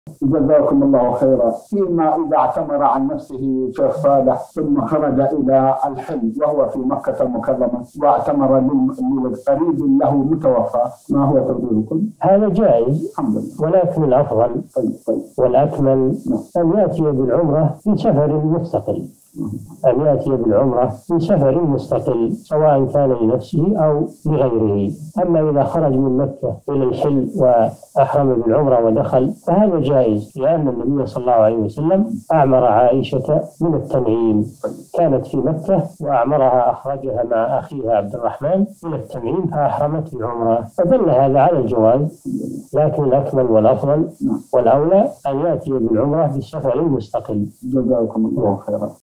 ما حكم إذا اعتمر الإنسان عن نفسه ثم خرج إلى الحل وهو في مكة واعتمر لقريب له متوفى؟ صالح الفوزان
نص السؤال: فيما إذا اعتمر عن نفسه يا شيخ صالح ثم خرج إلى الحل وهو في مكة المكرمة واعتمر لقريب له متوفى، ما هو توجيهكم؟
السيرة الذاتية :
للاشتراك في قناة الشيخ صالح الفوزان

رئيس الفريق
لمشروع كبار العلماء